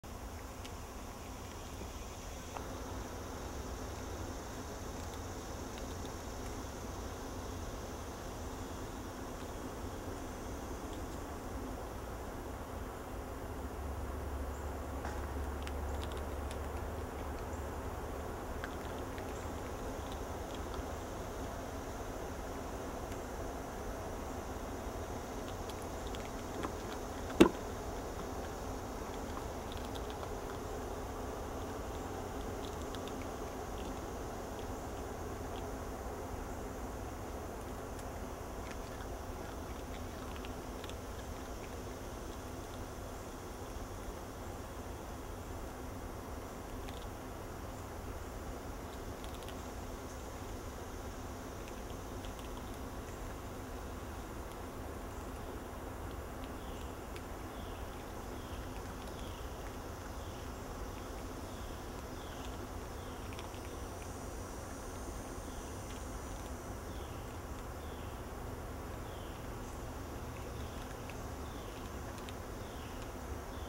Watch this lemur eat a biscuit for 60 seconds
Blue-eyed black lemur at the Duke Lemur Center

Even the free-ranging lemurs at the Duke Lemur Center receive primate chow, a nutritious supplement to the wild fruits and leaves they can forage on in our forested enclosures. They receive chow every day, and they like pretty well, even though it can be a little crunchy.